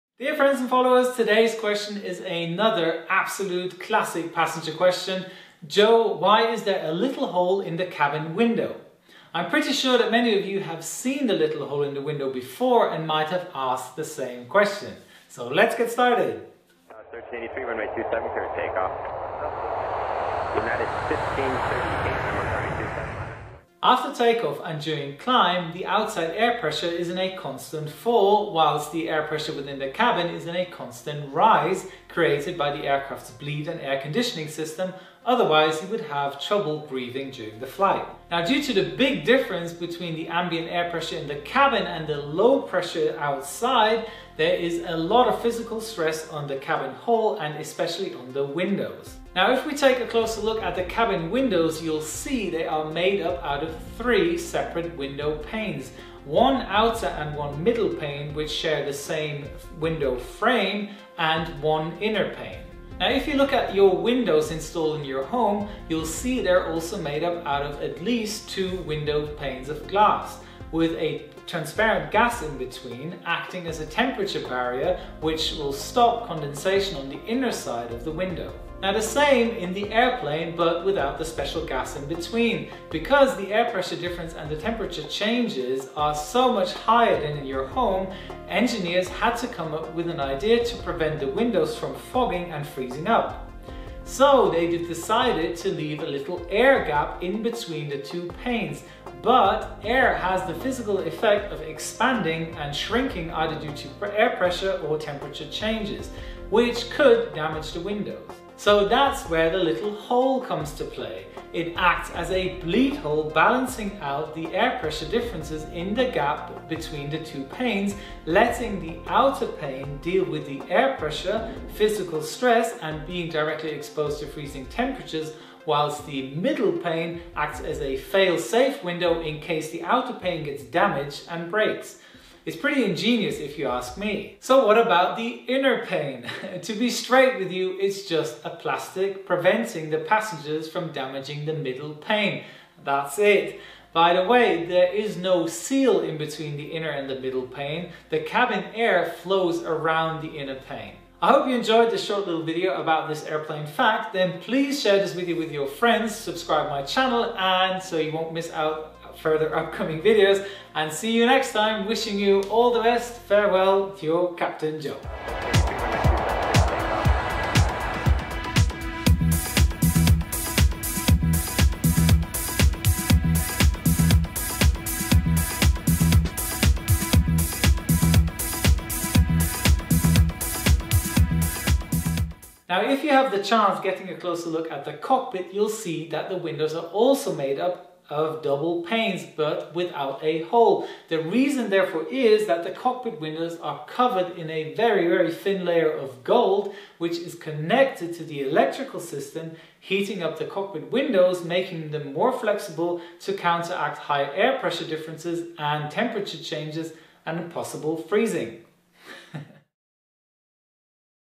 Why is there a HOLE in AIRPLANE windows?
After take-off and during climb, the outside air pressure is in a constant fall, whilst the air pressure within the cabin is in a constant rise created by the aircraft's bleed and air conditioning system, otherwise you would have trouble breathing during the flight. Due to the big difference between the ambient pressure in the cabin and the low pressure outside, there´s is a lot of physical stress on the cabin hull and especially on the windows.

Now if we take a closer look at the cabin windows you´ll see they are made up out of three separate window panes. One outer and middle pane which share the same window frame and one inner pane.

Now if you look at your windows installed in your home you´ll see they are also made up out of at least two panes of glass, with a transparent gas in between them, acting as a temperature barrier which stops condensation on the inner side of the window.

Same in the airplane but without the special gas in between. Because the air pressure difference and temperature variations are far much higher then in your home, engineers had to come up with and idea to prevent the windows from fogging and freezing up. So they´ve decided to leave an air gap in between the two panes. But air has the physical effect of expanding and shrinking either due to air pressure or temperature changes, which could damage the windows.

See the rest of the video for more information!